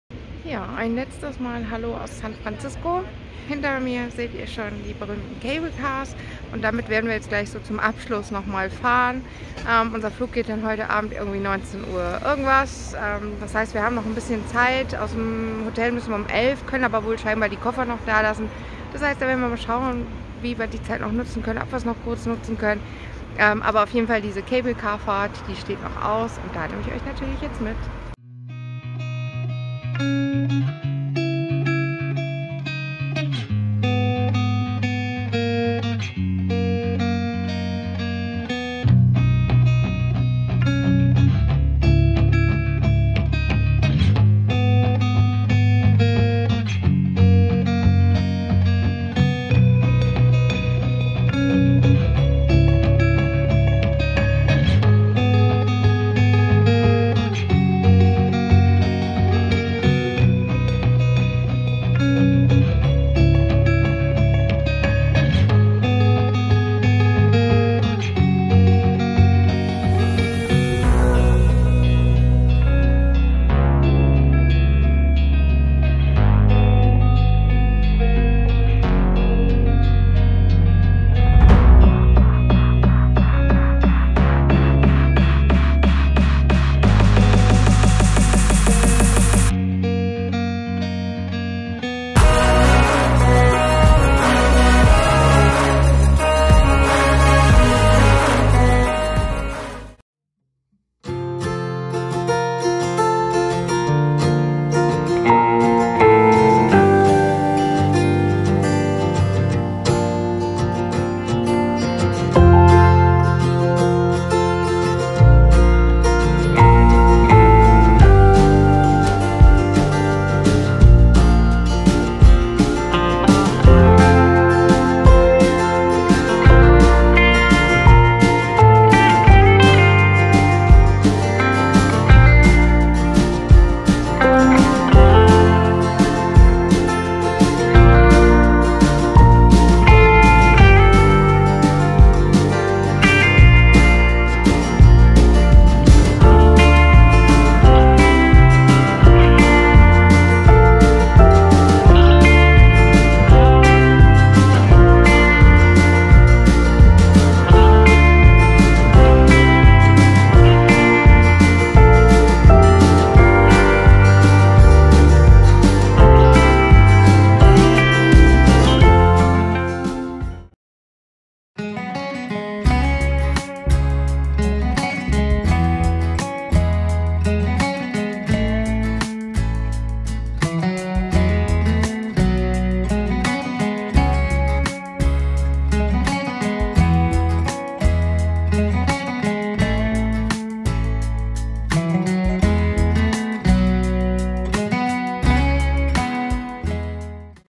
Vlog 11 Die letzten Stunden in San Francisco
Nun war er da: Der Tag der Abreise. Eine wundervolle Reise ging viel, viel zu schnell zu Ende. Doch da der Rückflug erst abends ging, konnten wir noch ein richtiges Highlight in San Francisco genießen und auch noch ein bisschen bummeln.